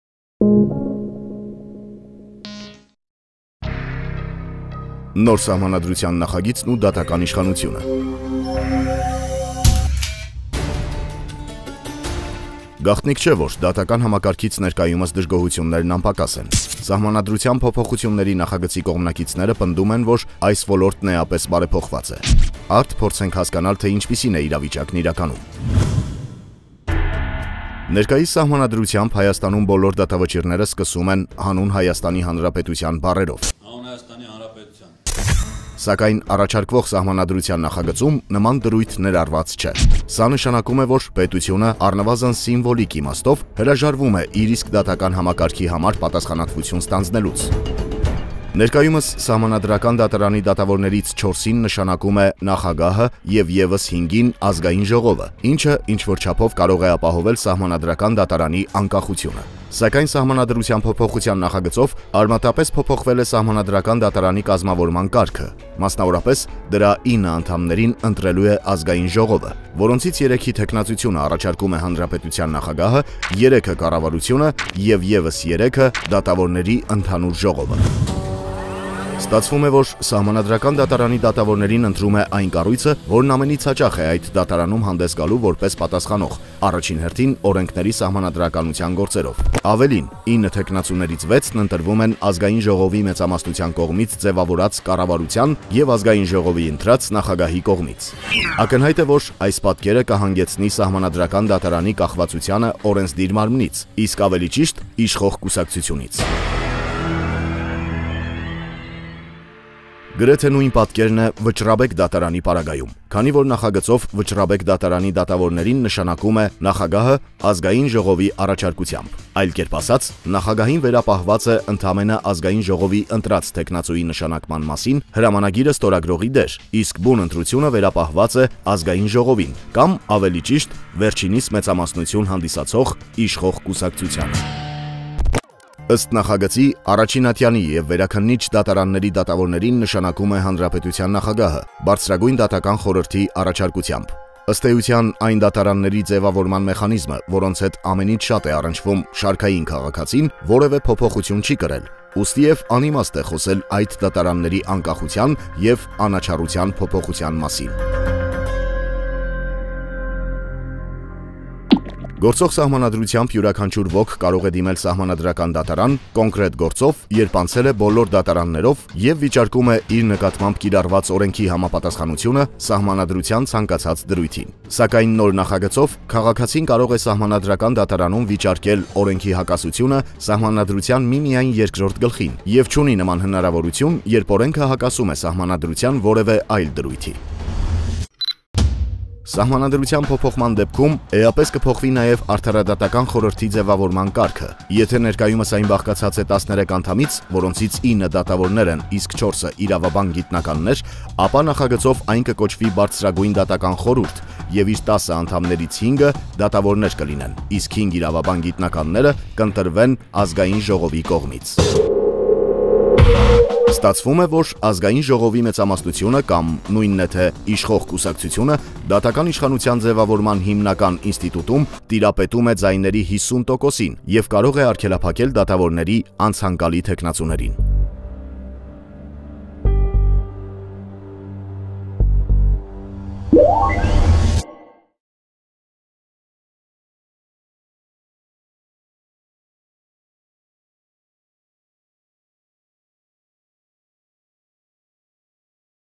ԴԱՏԱԿԱՆ ԻՇԽԱՆՈՒԹՅՈՒՆԸ / Judicial power
Հոլովակում ներկայացվում են օրենքի նոր նախագծի շրջանակներում դատական իշխանության դրությունը, դրանում կատարված փոփոխությունները:

The video presents the state of Judicial Power within the scope of the new draft law of the Republic of Armenia and the changes made in Judicial Power.